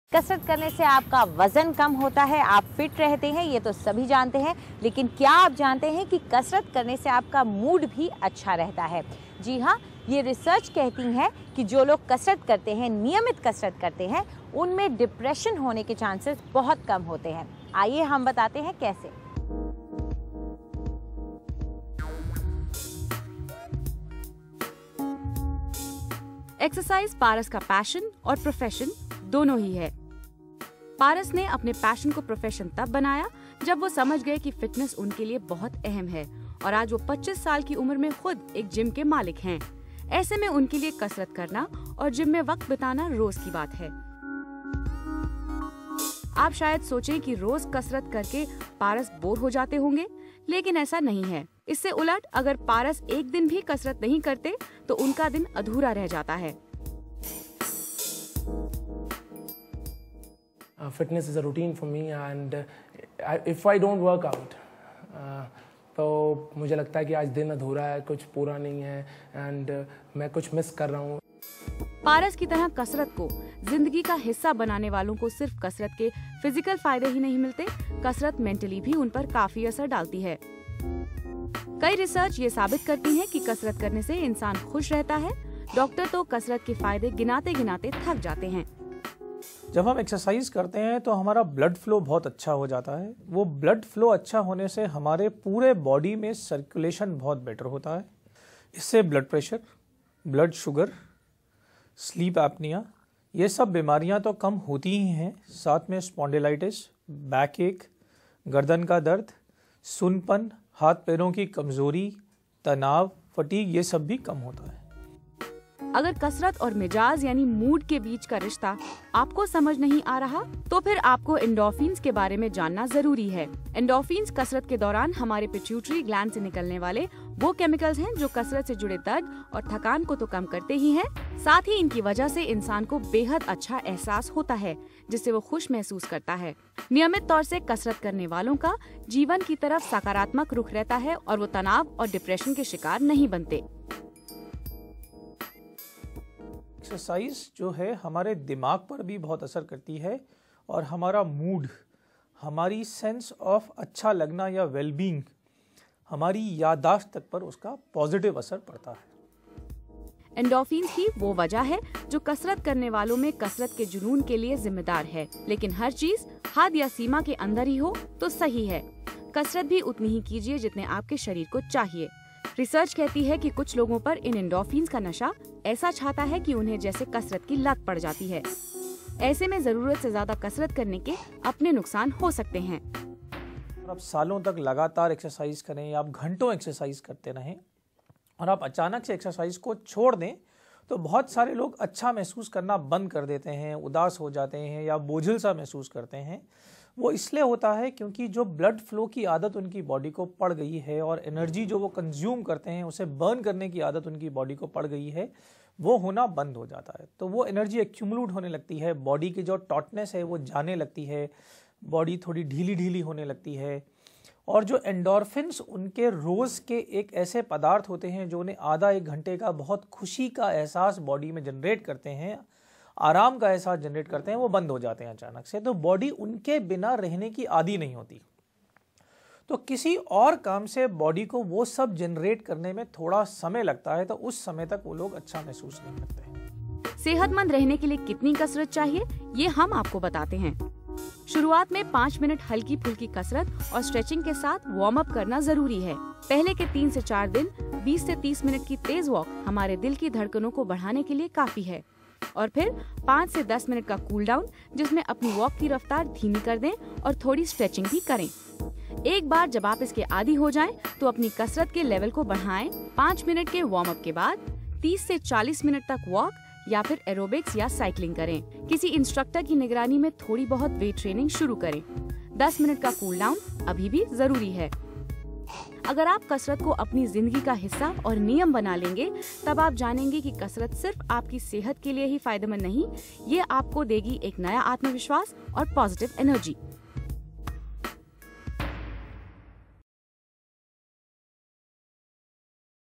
कसरत असर डालती है मूड पर
जो लोग नियमित रूप से कसरत करते हैं उनमें डिप्रेशन होने की आशंका बहुत कम होती है।